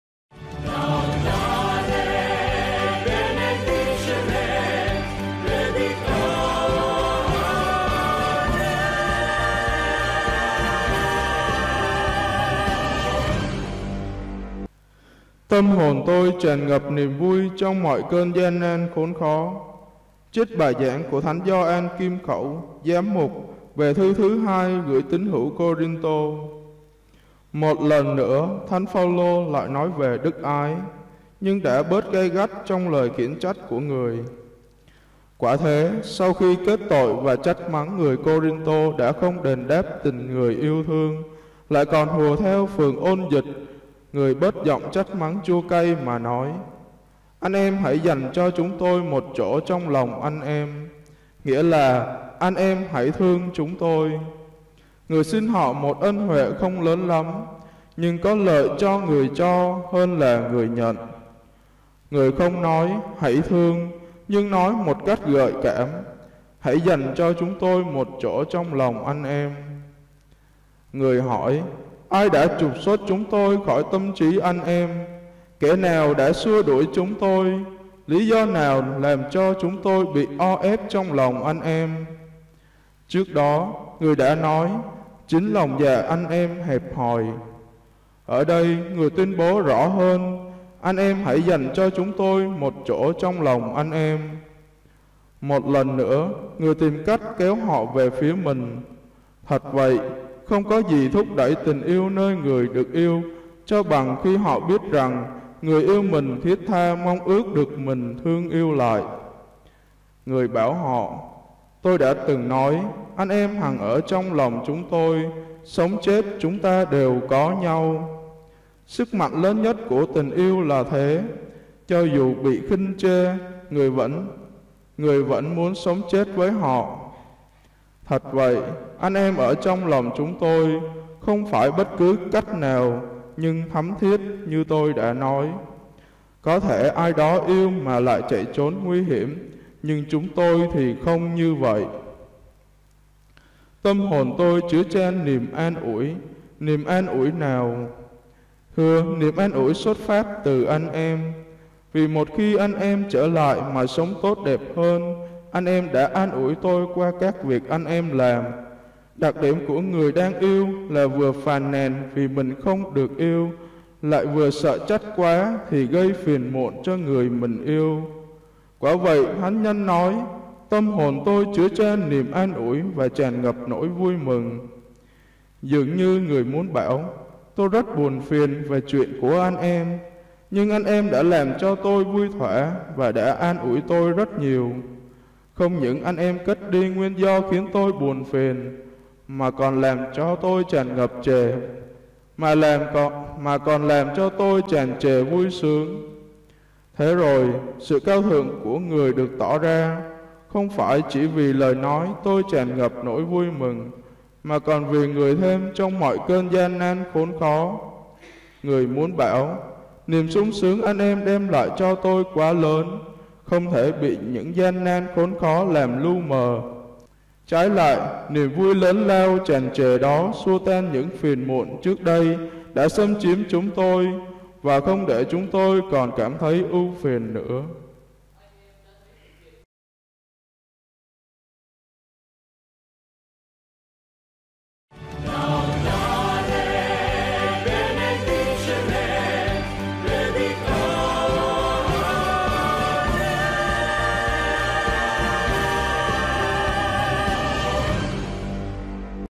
TÂM HỒN TÔI TRÀN NGẬP NIỀM VUI TRONG MỌI NƠI GIAN NAN KHỐN KHÓ
Trích bài giảng của thánh Gioan Kim Khẩu, giám mục, về thư thứ hai gửi tín hữu Côrintô.
Một lần nữa, thánh Phaolô lại nói về đức ái, nhưng đã bớt gay gắt trong lời khiển trách của người. Quả thế, sau khi kết tội và trách mắng người Côrintô đã không đền đáp tình người yêu thương, lại còn hùa theo phường ôn dịch, người bớt giọng trách mắng chua cay mà nói: Anh em hãy dành cho chúng tôi một chỗ trong lòng anh em, nghĩa là “anh em hãy thương chúng tôi”. Người xin họ một ân huệ không lớn lắm, nhưng có lợi cho người cho hơn là người nhận. Người không nói “hãy thương”, nhưng nói một cách gợi cảm hãy dành cho chúng tôi một chỗ trong lòng anh em.
Người hỏi: “Ai đã trục xuất chúng tôi ra khỏi tâm trí anh em? Kẻ nào đã xua đuổi chúng tôi? Lý do nào làm cho chúng tôi bị o ép trong lòng anh em?” Trước đó người đã nói: Chính lòng dạ anh em hẹp hòi. Ở đây người tuyên bố rõ hơn: Anh em hãy dành cho chúng tôi một chỗ trong lòng anh em. Một lần nữa, người tìm cách kéo họ về với mình. Thật vậy, không có gì thúc đẩy tình yêu nơi người được yêu cho bằng khi họ biết rằng người yêu mình thiết tha mong ước được mình yêu thương lại.
  Người bảo họ: Tôi đã từng nói: anh em hằng ở trong lòng chúng tôi, sống chết chúng ta đều có nhau. Sức mạnh lớn nhất của tình yêu là thế: cho dù bị khinh chê, người vẫn muốn sống chết với họ. Thật vậy, anh em hằng ở trong lòng chúng tôi, không phải bất cứ cách nào, nhưng thắm thiết, như tôi đã nói. Có thể ai đó yêu mà chạy trốn nguy hiểm; nhưng chúng tôi thì không như vậy.
Tâm hồn tôi chứa chan niềm an ủi. Niềm an ủi nào? Thưa niềm an ủi xuất phát từ anh em. Vì một khi anh em trở lại mà sống tốt đẹp hơn, anh em đã an ủi tôi qua các việc anh em làm. Đặc điềm của người đang yêu là phàn nàn vì mình không được yêu, lại sợ trách quá thì gây phiền muộn cho người mình yêu. Vì vậy thánh nhân nói: Tâm hồn tôi chứa chan niềm an ủi, và tràn ngập nỗi vui mừng.
Dường như người muốn bảo: “Tôi rất buồn phiền về chuyện của anh em; nhưng anh em đã làm cho tôi vui thoả và đã an ủi tôi rất nhiều. Không những anh em cất đi nguyên do khiến tôi buồn phiền, mà còn làm cho tôi tràn trề vui sướng.”
Thế rồi, sự cao thượng của người được tỏ ra, không phải chỉ vì lời nói Tôi tràn ngập nỗi vui mừng, mà còn vì người thêm trong mọi cơn gian nan khốn khó. Người muốn bảo: niềm sung sướng anh em đem lại cho tôi quá lớn, không thể bị những gian nan khốn khó làm lu mờ; trái lại, niềm vui lớn lao tràn trề đó xua tan những phiền muộn trước đây đã xâm chiếm chúng tôi, và không để chúng tôi còn cảm thấy ưu phiền nữa.
Mong thay